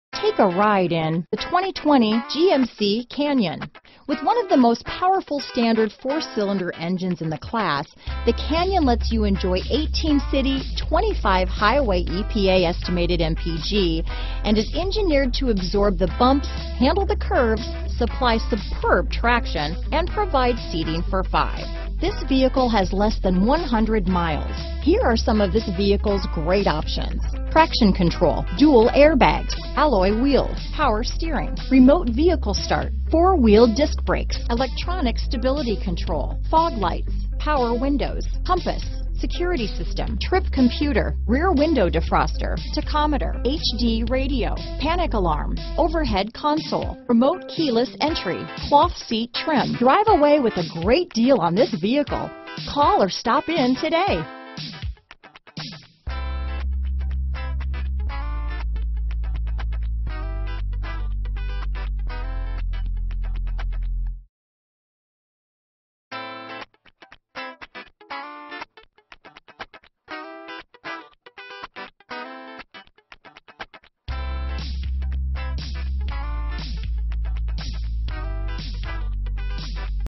2020 GMC Canyon Corpus Christi, Kingsville, Alice, San Antonio, Robstown, TX 1184080
Sky New 2020 GMC Canyon available in Robstown, Texas at Beck & Masten GMC Buick Coastal Bend. Servicing the Corpus Christi, Kingsville, Alice, San Antonio, TX area.

Sky 2020 GMC Canyon SLE1 RWD 6-Speed Automatic 2.5L I4 DI DOHC VVTbrbr20/26 City/Highway MPG Price includes: $2750 - GM Consumer Cash Program . Exp. 04/30/2020